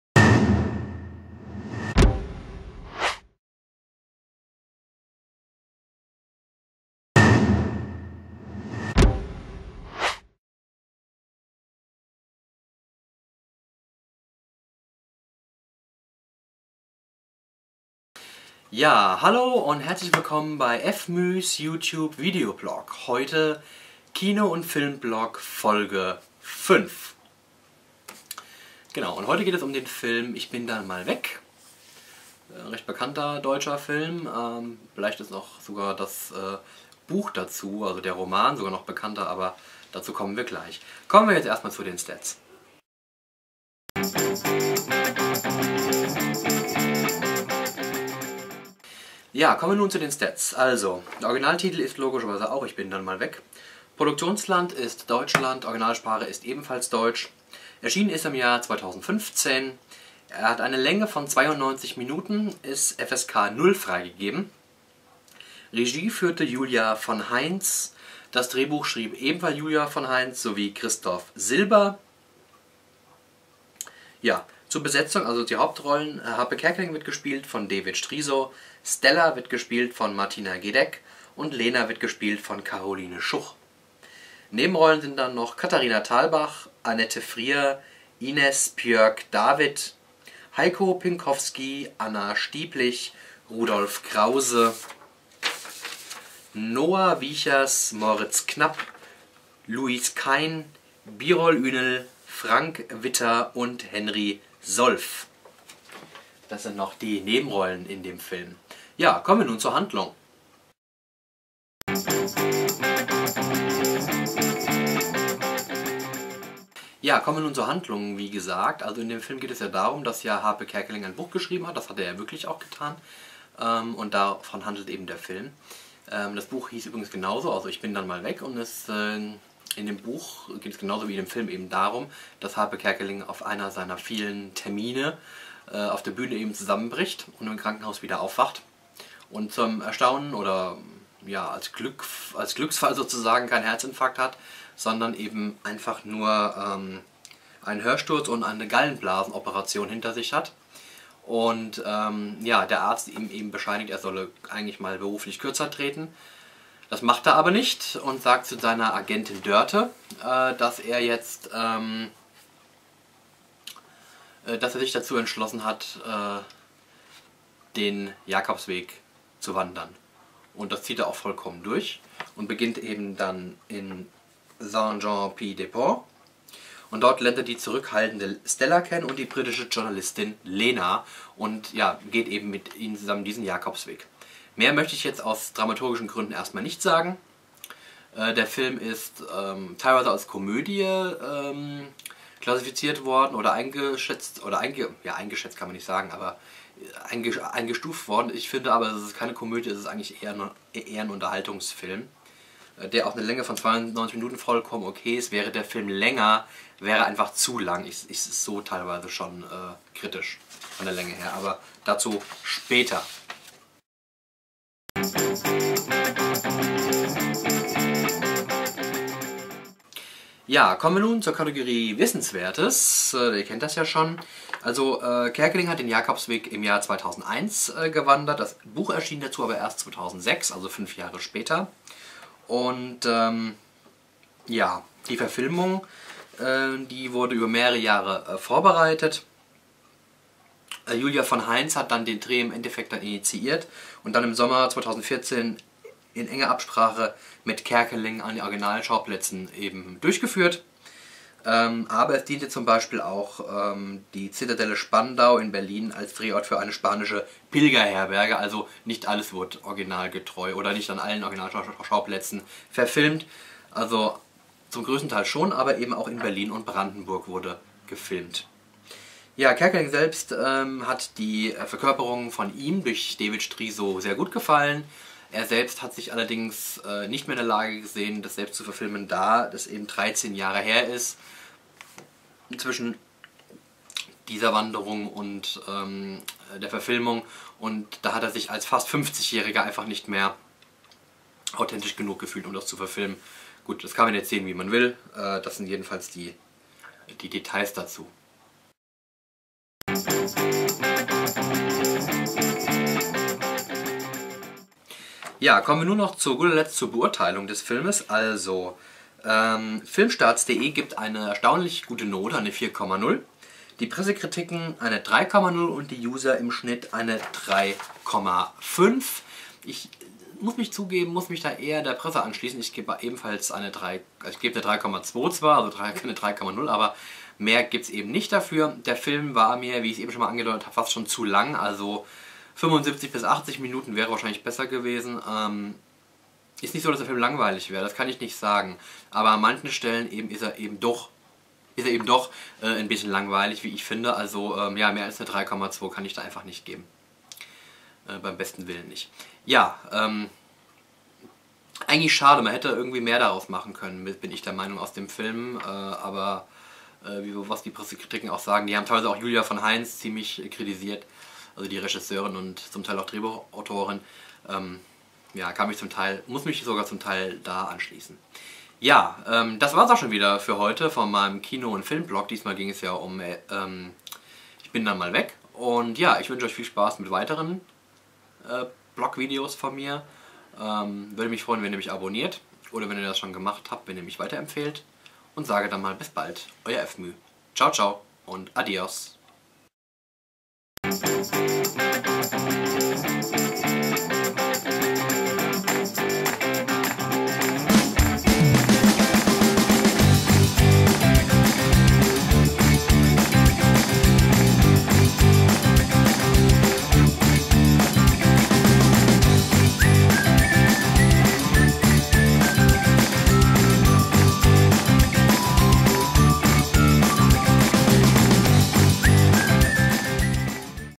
Kino und Film Blog: Folge 5 - Ich bin dann mal weg
Die Folge 5 meines Kino und Film-Blogs ist online. Heute geht es um den Film "Ich bi  dann mal weg"